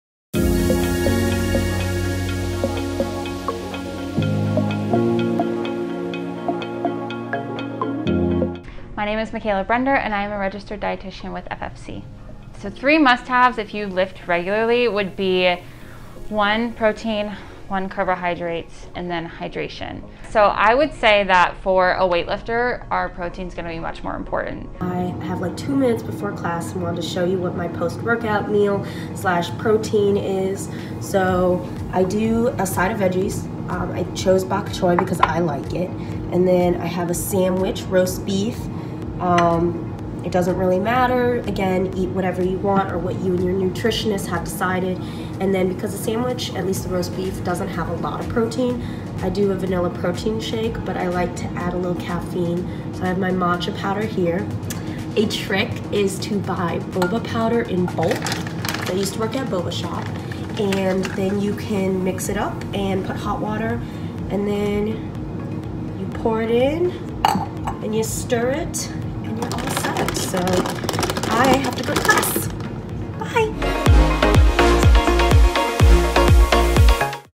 Week 3: Learn How Emily Is Adjusting Her Nutrition To Help With Her Goals
During week 3, Emily meets with an FFC Registered Dietitian to learn how to adjust her nutrition as she continues with her weight-lifting workouts.